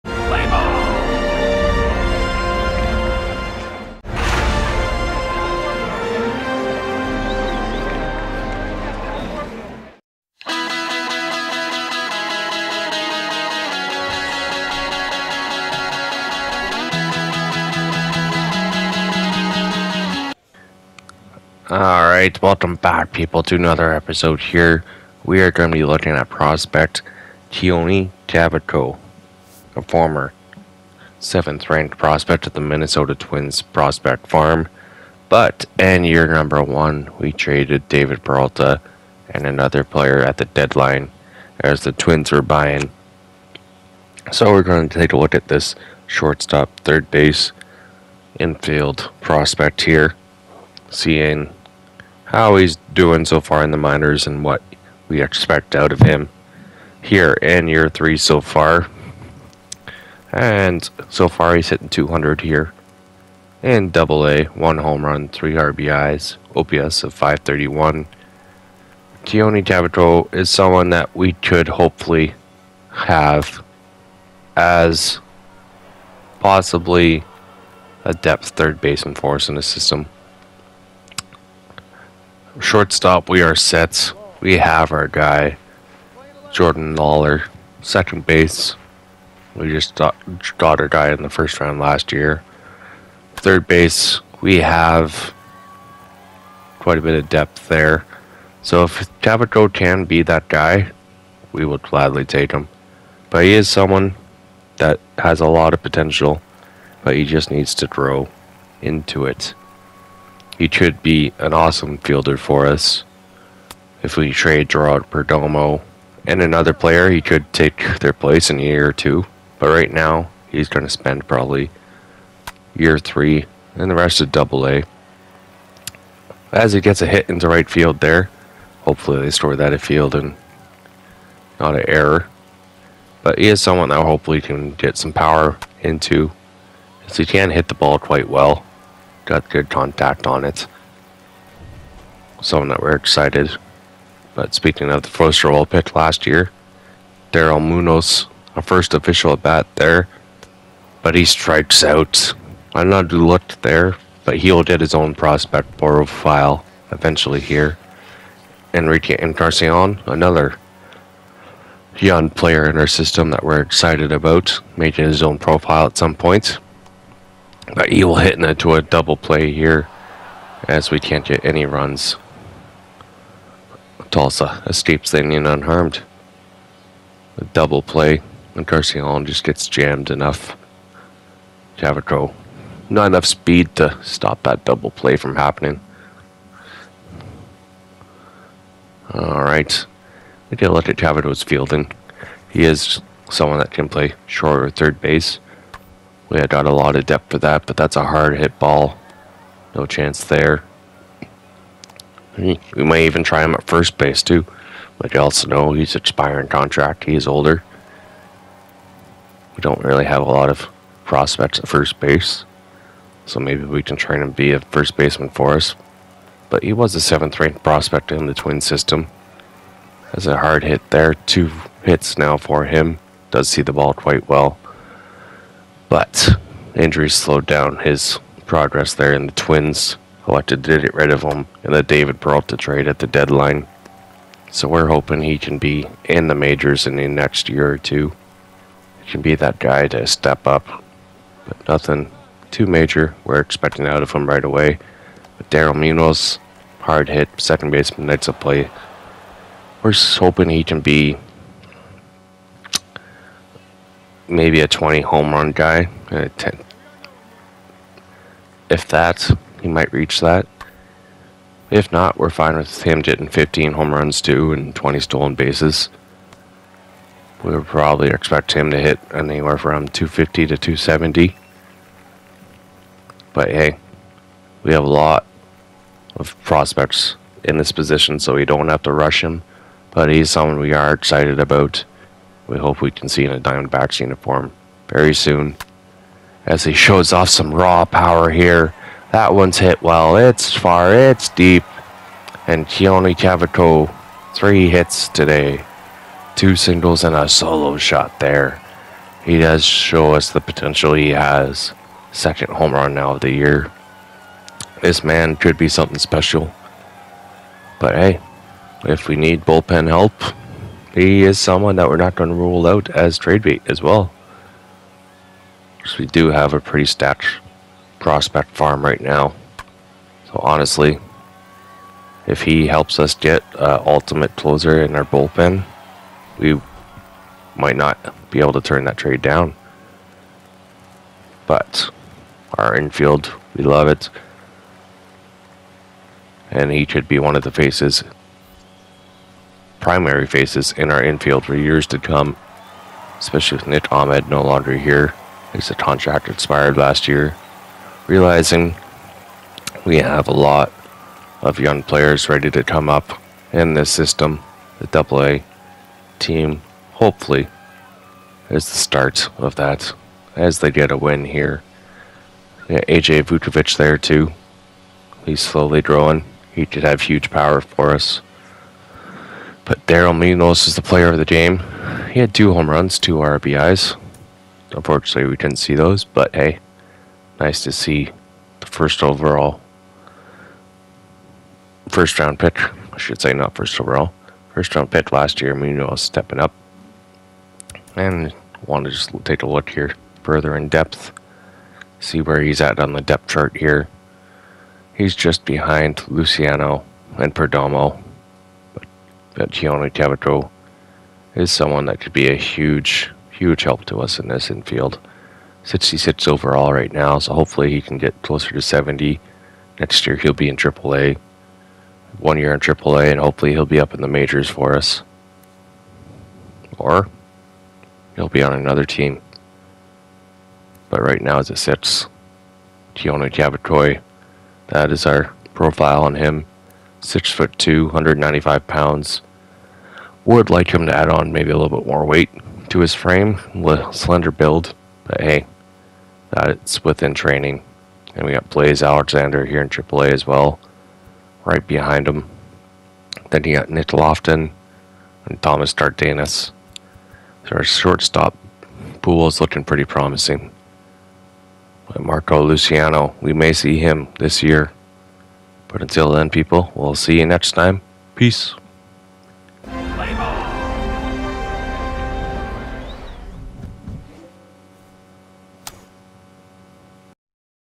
Arizona Diamondbacks Franchise Rebuild Prospect profile- Keoni Cavaco
taking a closer look at prospect Keoni Cavaco the player we got in the david peralta trade. Cavaco is someone we are trying to groom to be utility infeilder with some pop in his bat.  as long as he stays healthy we could  see him in the MLB in two years